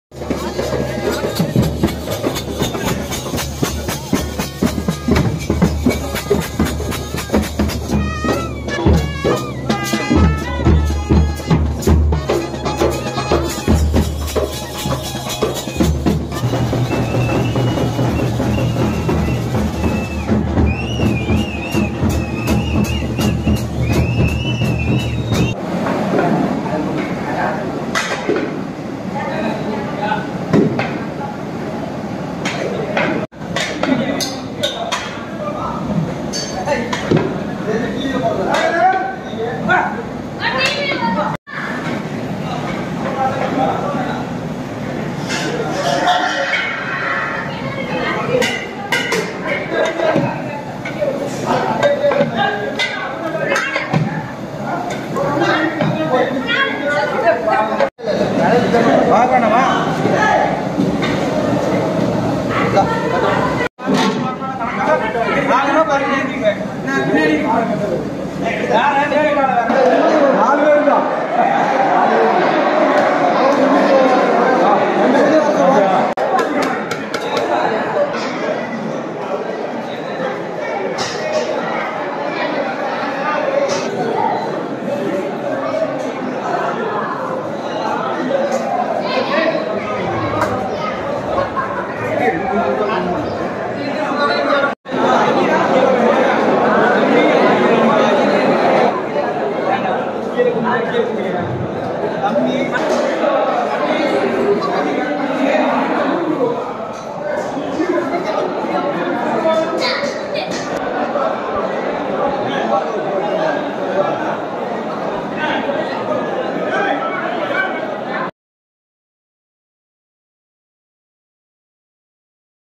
ஜெயிக்க வைத்த மக்களுக்கு கறி விருந்து வைத்த சேர்மன்!| ETV Bharat Tamilnadu
நாமக்கல் அருகே தேர்தலில் வெற்றி பெறுவதற்கு காரணமாக இருந்த ஊர் மக்களுக்கு பேரூராட்சித் தலைவர் தடபுடலாக கறி விருந்து வைத்து அசத்தியுள்ளார்.